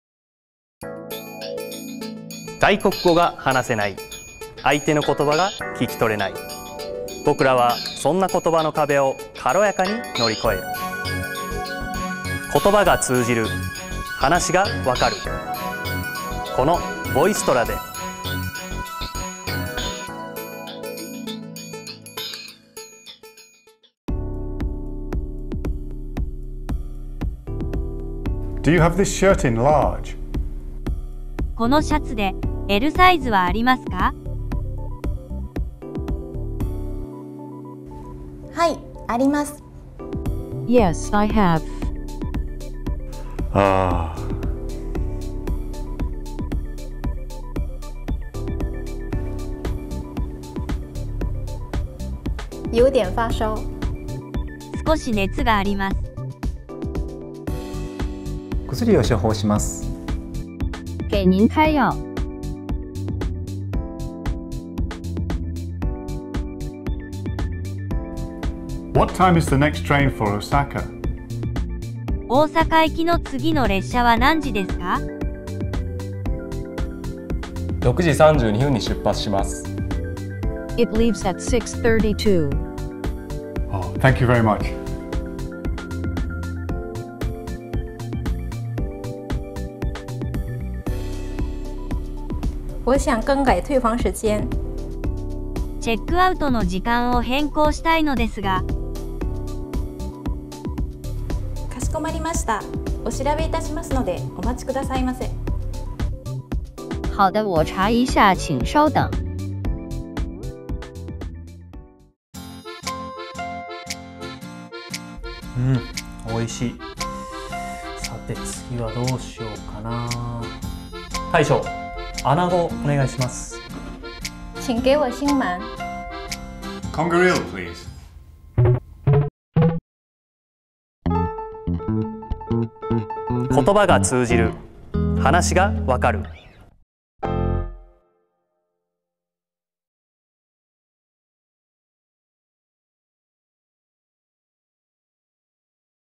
VoiceTra Video Guide: Introduction to VoiceTra
This video explains expected situations in which VoiceTra (a multilingual speech translation application for smartphones researched and developed by NICT) is helpful.